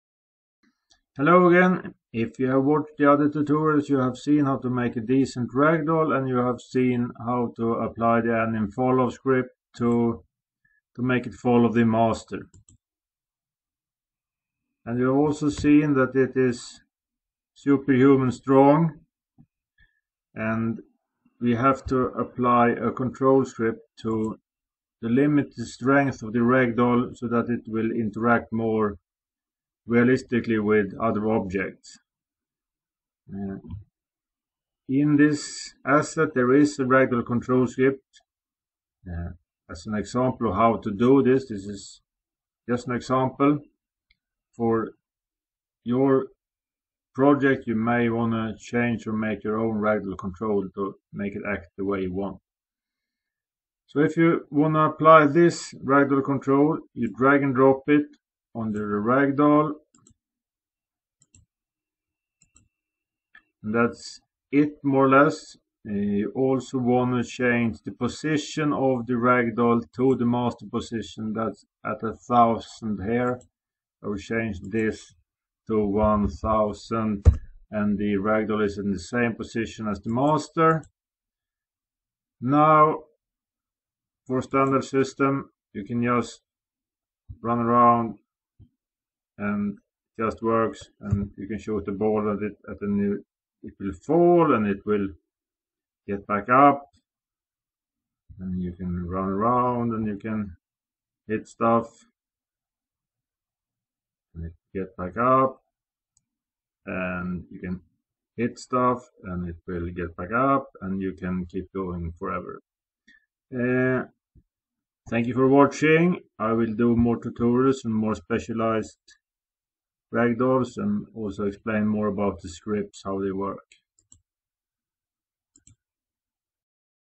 How to apply the RagdollControl script
The RagdollControl moderates the ragdoll´s strength according to the context.